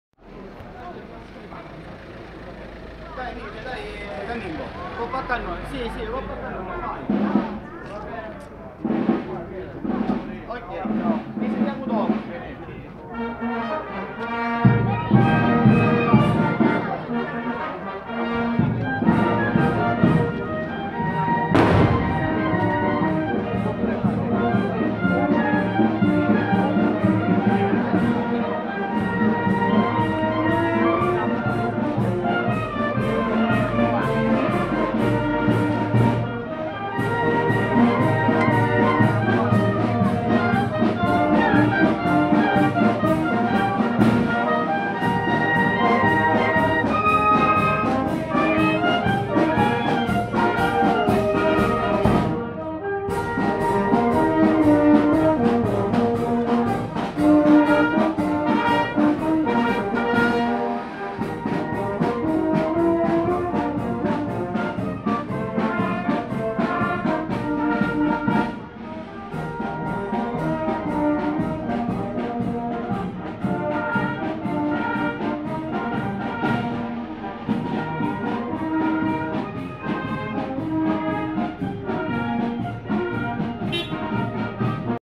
ACIBONACCORSI,15 LUGLIO 2018 - FESTA DI S.STEFANO WP 20180715 017
Uscita dell Fercolo dalla casa a vara